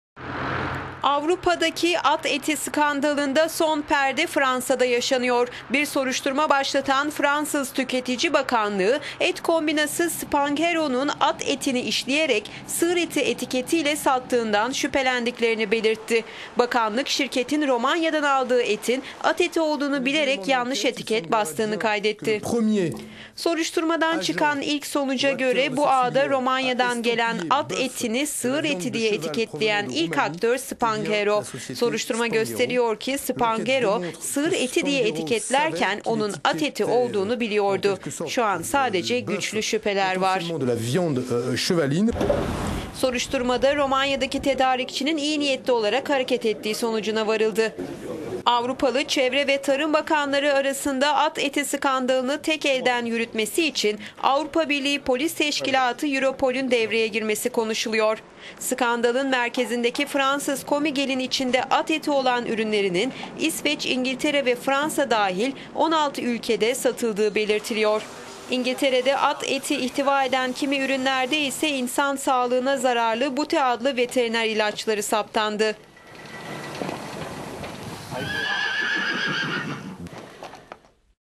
Fransız et kombinası at etini 'sığır eti' diye etiketlemiş
Avrupa'daki at eti skandalında son perde Fransa'da yaşanıyor. Bir soruşturma başlatan Fransız Tüketici Bakanlığı, et kombinası Spanghero'nun at etini işlereyek, 'sığır eti' etiketiyle sattığı konusunda ciddi şüphelerinin olduğunu belirtti. Fransa Tüketici Bakanı Benoit Hamon, şirketin Romanya'dan aldığı etin, at eti olduğunu bilerek yanlış etiket bastığını kaydetti

"Soruşturmadan çıkan sonuca göre bu ağda Romanya'dan gelen at etini 'sığır eti' diye etiketleyen ilk aktör Spanghero. Soruşturma gösteriyor ki Spanghero sığır eti diye etiketlerken onun at eti olduğunu biliyordu. Şu an sadece çok güçlü şüpheler var."

Soruşturmada Romanya'daki tedarikçinin ise iyi niyetli olarak hareket ettiği sonucuna varıldı. Fransa Tarım Bakanlığı, sözkonusu şirketin faaliyet izninin iptal edilebileceğini belirtti.

Avrupalı çevre ve tarım bakanları arasında at eti skandalını tek elden yürütmesi için Avrupa Birliği polis teşkilatı Europol'ün devreye girmesi konuşuluyor.

Skandalın merkezindeki Fransız Comigel'in içinde at eti olan ürünlerinin İsveç, İngiltere ve Fransa dahil 16 ülkede satıldığı belirtiliyor.

İngiltere'deki at eti ihtiva eden kimi ürünlerin ise insan sağlığına zararlı 'phenylbutazone' ya da diğer adıyla 'bute' adlı veteriner ilaçlarını içerdiği saptandı.